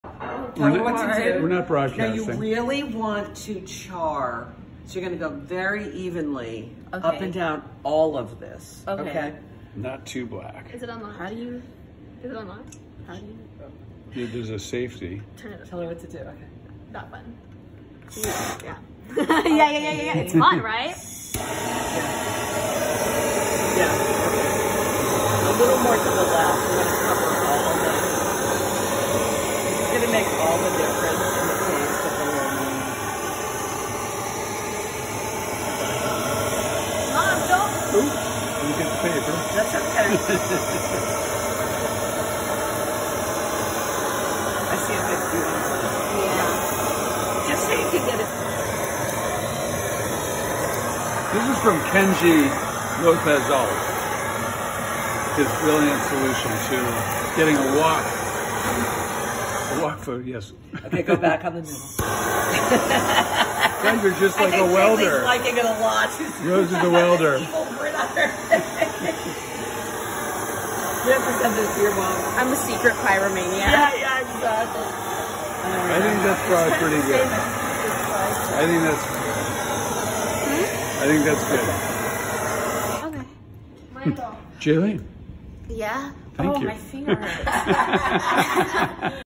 More blowtorch lo mein
We love this dish so much (thank you Kenji!) I got ann a real blow torch. A blow torch in the kitchen also is great to use on roasts, heating the surface before roasting for an extra browned crust. Here Annabelle's pal Jaileen weilds the torch on fresh fettuccini, shiitakes and vegetables. Great dish, recipe on my site, ruhlman.com